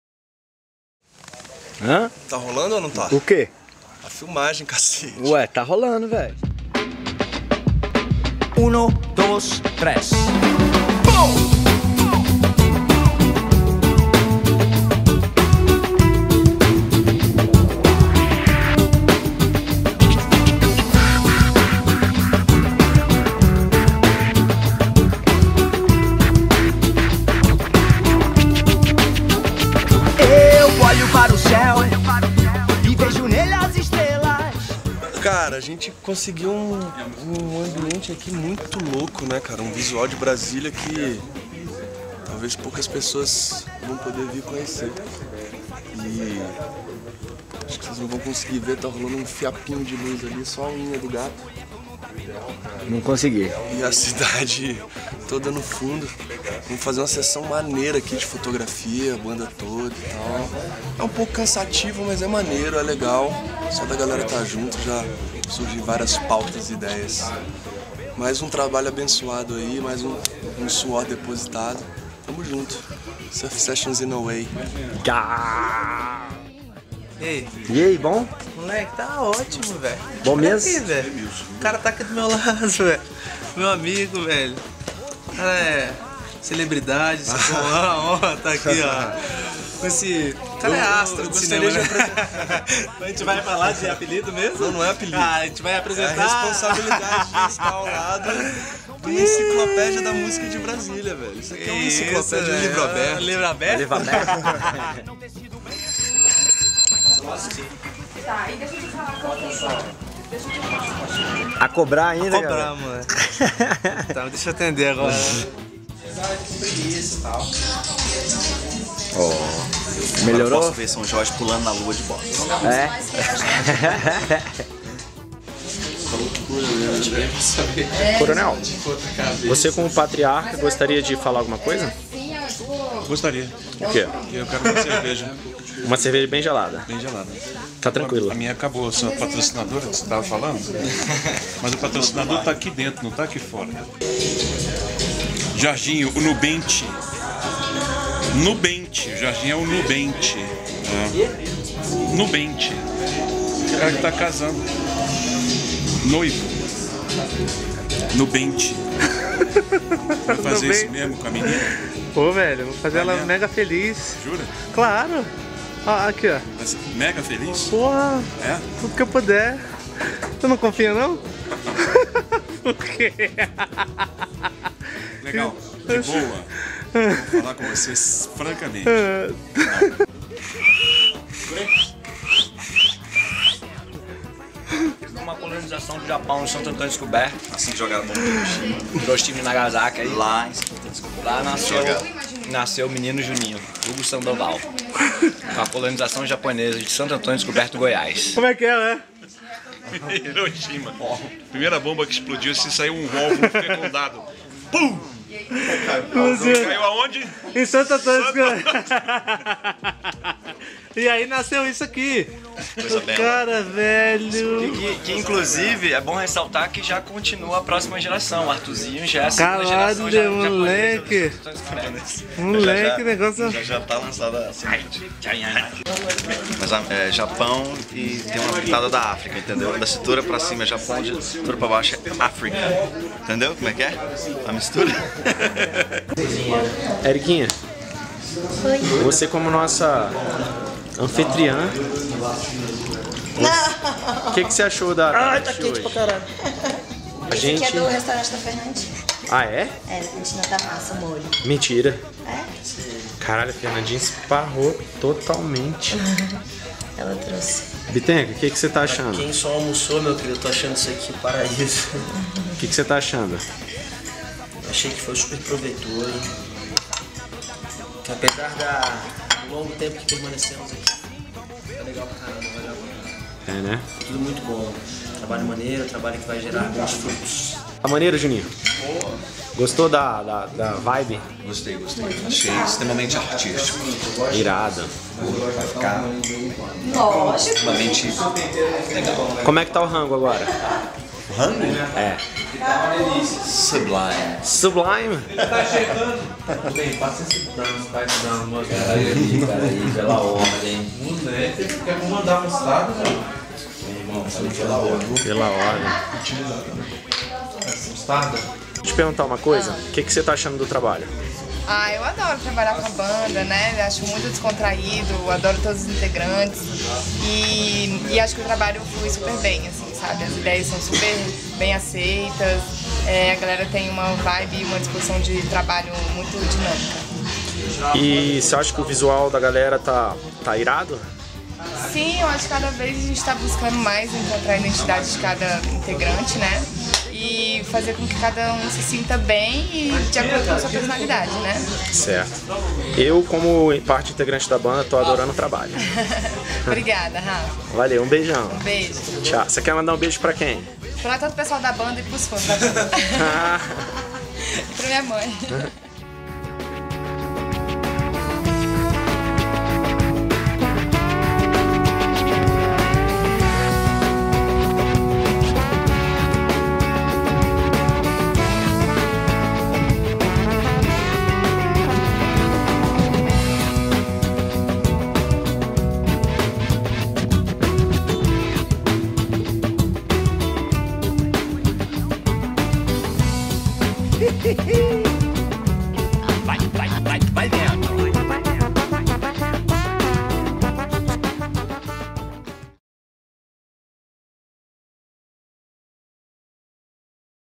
Ensaio Fotográfico com a Surf Sessions - Set/2012
Um making off do Ensaio Fotográfico realizado em setembro de 2012, em Brasília!
Para conferir as fotos, acesse nosso site ou página no FaceBook

Produção:

Ricardo Reis
Natália Ayres
Kaká Lopes

Styling de moda:

Natália Ayres

Foto:

Ricardo Reis

A banda Surf Sessions: Rafa Monte Rosa, Felipe Bittencourt, Renato Azambuja, Felipe Maguin, Júnior Fernandes e Jorge Bittar